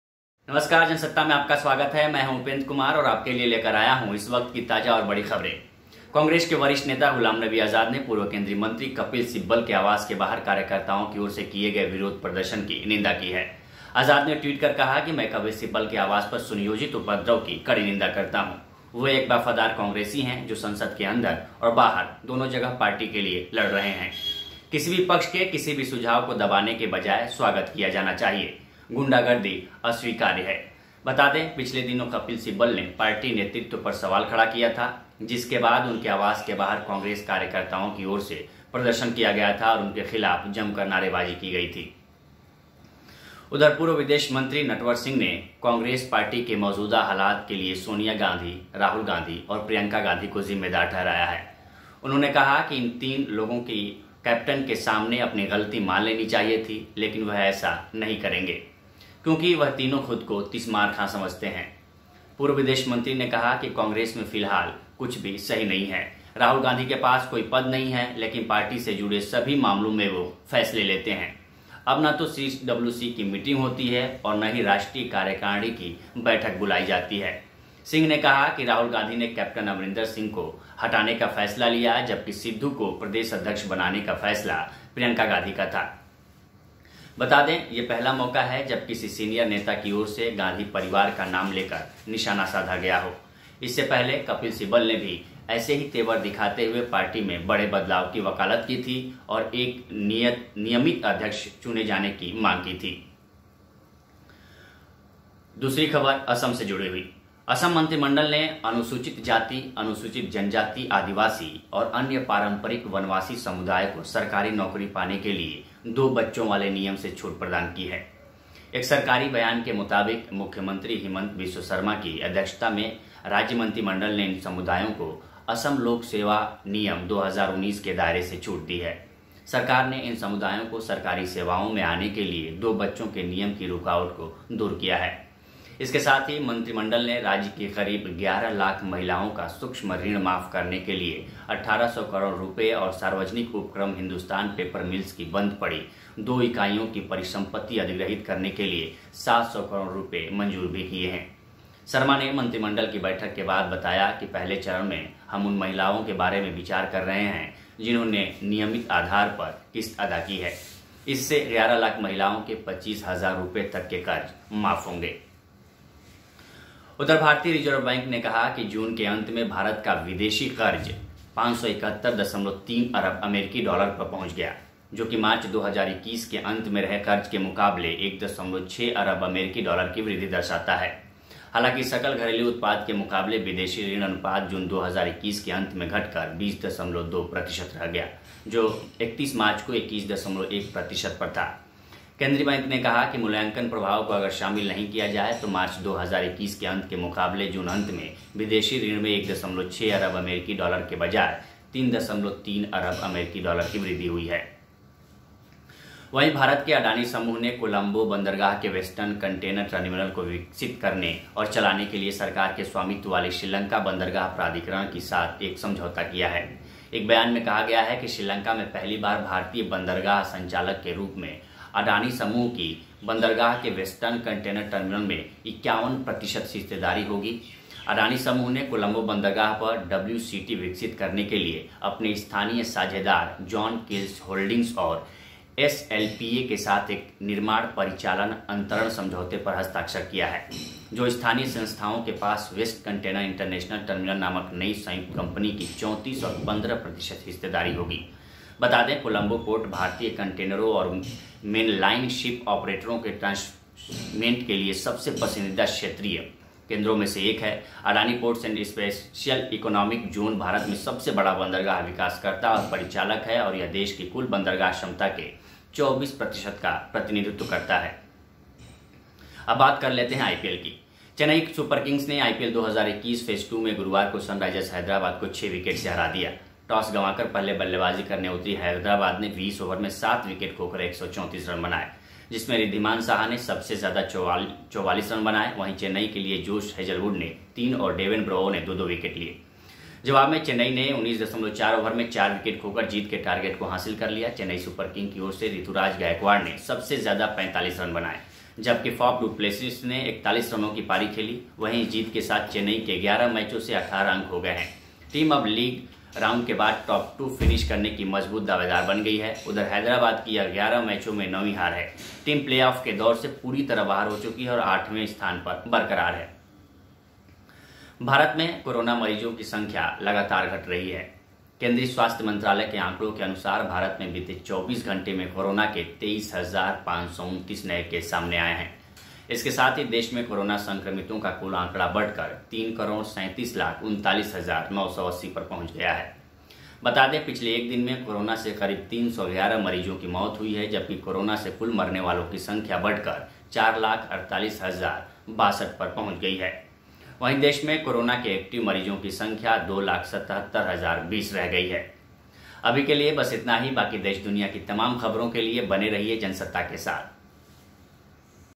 कपिल सिब्बल के आवास पर कार्यकर्ताओं का विरोध निंदनीय, बोले गुलाम नबी आजाद – गुंडागर्दी अस्वीकार्य
कांग्रेस के वरिष्ठ नेता गुलाम नबी आजाद ने पूर्व केंद्रीय मंत्री कपिल सिब्बल के आवास के बाहर कार्यकर्ताओं की ओर से किए गए विरोध प्रदर्शन की निंदा की. आजाद ने ट्वीट कर कहा कि मैं कपिल सिब्बल के आवास पर सुनियोजित उपद्रव की कड़ी निंदा करता हूं. वहीं, पूर्व विदेश मंत्री नटवर सिंह ने कांग्रेस पार्टी के मौजूदा हालात के लिए सोनिया गांधी, राहुल गांधी और प्रियंका गांधी को जिम्मेदार ठहराया है. उन्होंने कहा कि इन तीनों लोगों को कैप्टन के सामने अपनी गलती मान लेनी चाहिए थी, लेकिन वह ऐसा करेंगे नहीं, क्योंकि वह तीनों खुद को तीस मार खां समझते हैं. उधर, असम मंत्रिमंडल ने अनुसूचित जाति, अनुसूचित जनजाति, आदिवासी और अन्य पारंपरिक वनवासी समुदाय को सरकारी नौकरी पाने के लिए दो बच्चों वाले नियम से छूट प्रदान की है. एक सरकारी बयान के मुताबिक, मुख्यमंत्री हिमंत बिस्व सरमा की अध्यक्षता में राज्य मंत्रिमंडल ने इन समुदायों को असम लोक सेवा नियम, 2019 के दायरे से छूट दी है. Jansatta is one of the leading hindi news channel, which provides all the latest and trending news of india and around the world.